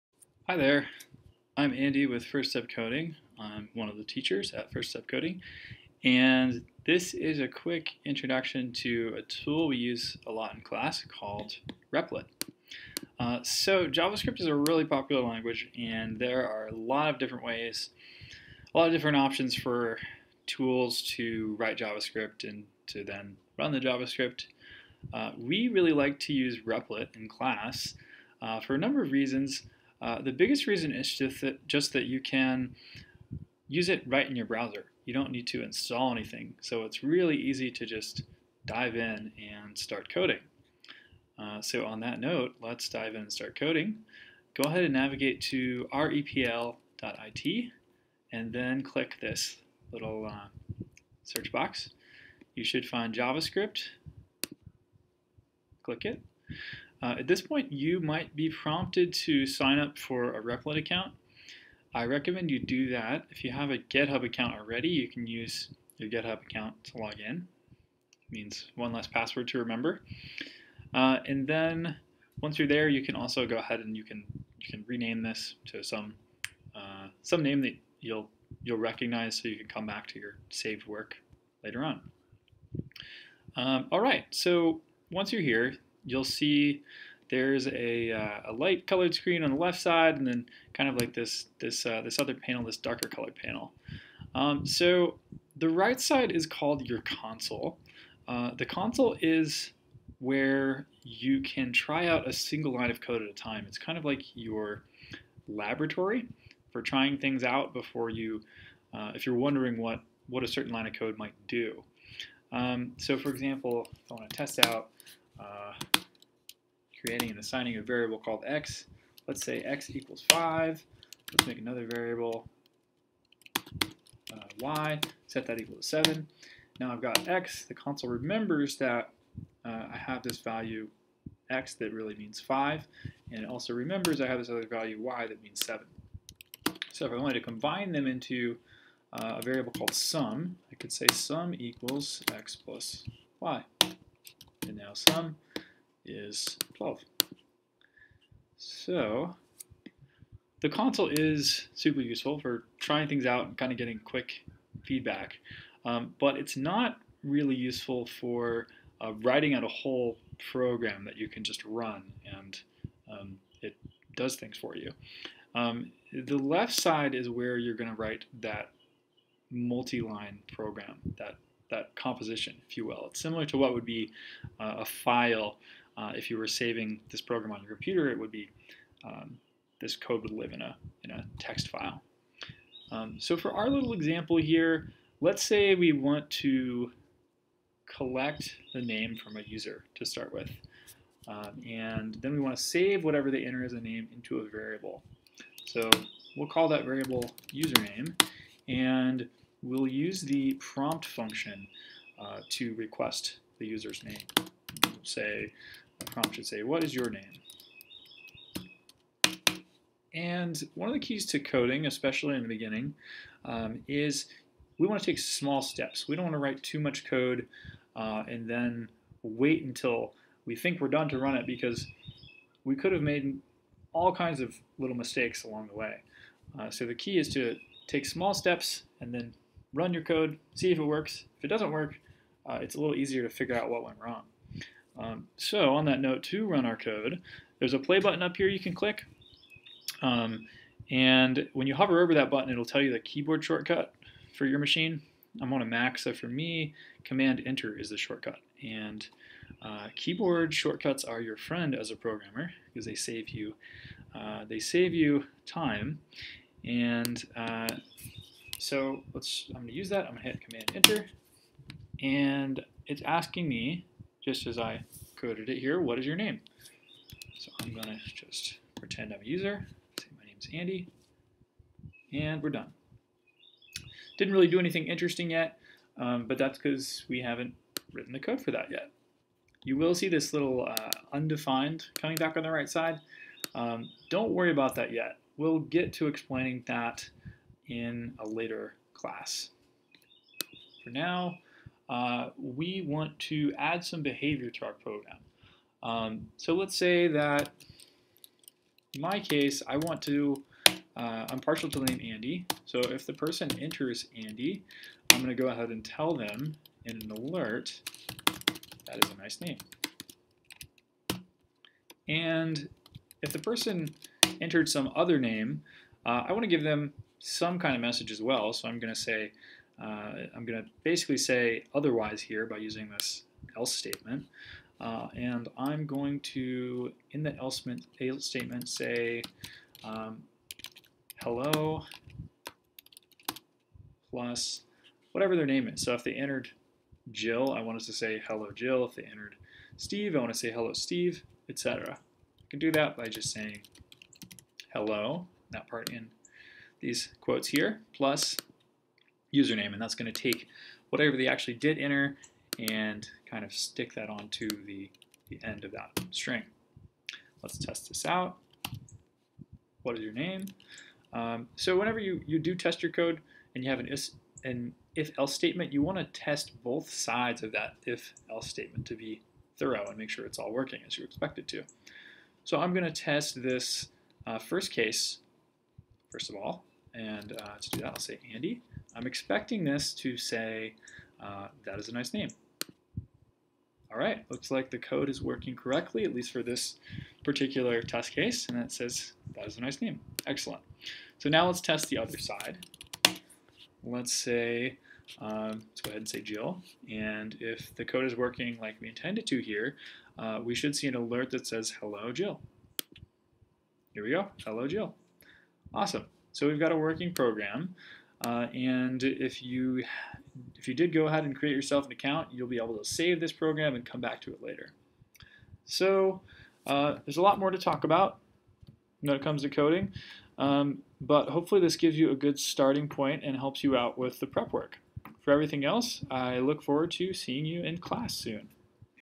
First Step Coding intro to Repl.it
Introduction to using Repl.it, a tool for writing and running JavaScript in your browser.

Update: Replit now uses Node when you create a new JavaScript Replit instead of "browser JavaScript". This means that the prompt function will not work as I show it. To be able to follow all of this tutorial, create an HTML Replit instead via this

Then select  the script.js file to work in (on the left) and the Console view on the right. Everything should then work for you like it does in this video.